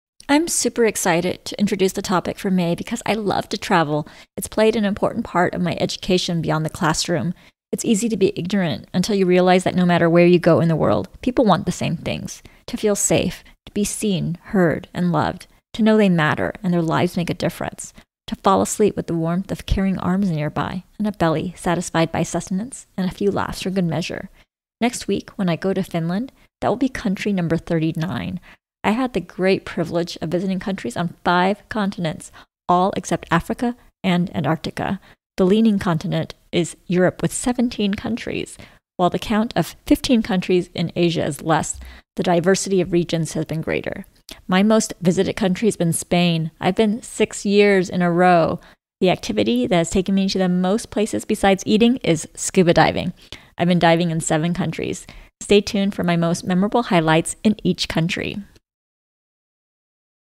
Episode 120 - Diversity And Travel
I’m super excited to introduce the topic for May because I love to travel. It’s played an important part of my education beyond the classroom. It’s easy to be ignorant until you realize that no matter where you go in the world people want the same things. To feel safe. To be seen, heard and loved. To know they matter and their lives make a difference. To fall asleep with the warmth of caring arms nearby and a belly satisfied by sustenance and a few laughs for good measure. Next week when i go to Finland, that will be country #39. I’ve had the great privilege of visiting countries on 5 continents, all except Africa and Antarctica. The leading continent is Europe with 17 countries.  While the count of 15 countries in Asia is less, the diversity of regions has been greater. My most visited country has been Spain, I’ve been six years in a row. The activity that has taken me to the most places besides eating is scuba diving, I’ve been diving in 7 countries. Want answers? I hope you’ve been enjoying it.